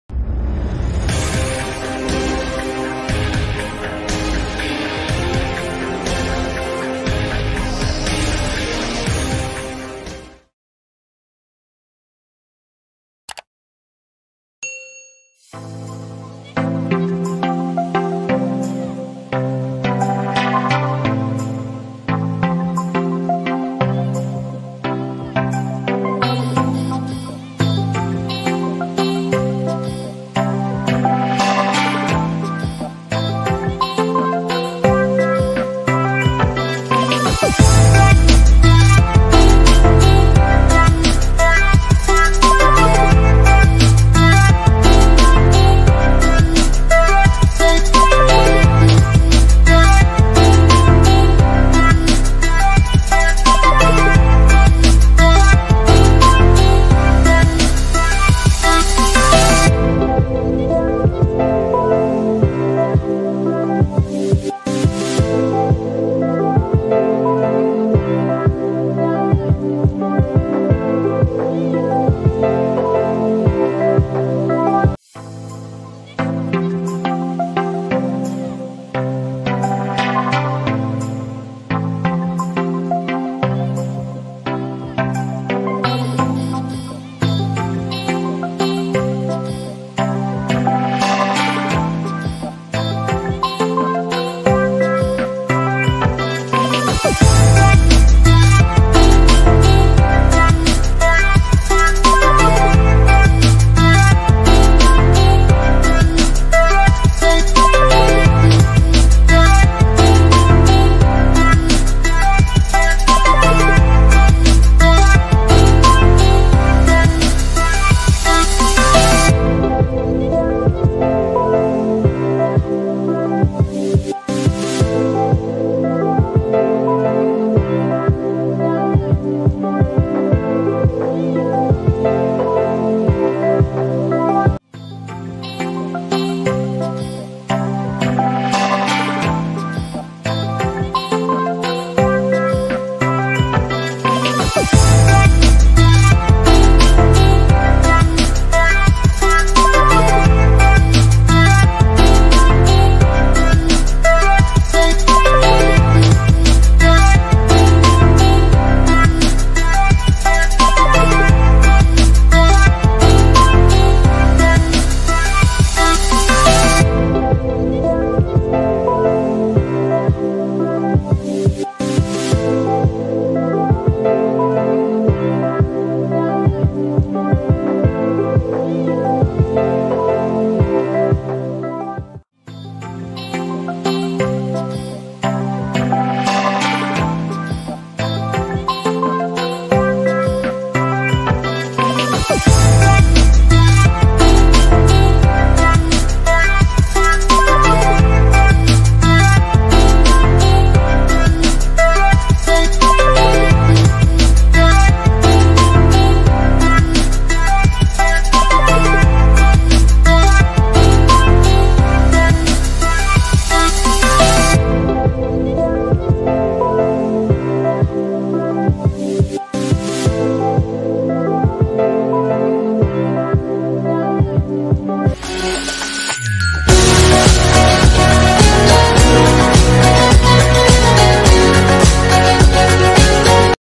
Review servis Jok Di Plered(Gonggo)vloger
slmat mononton vidio nya
saya mencoba review tukang servis jok daerah plered purwakarta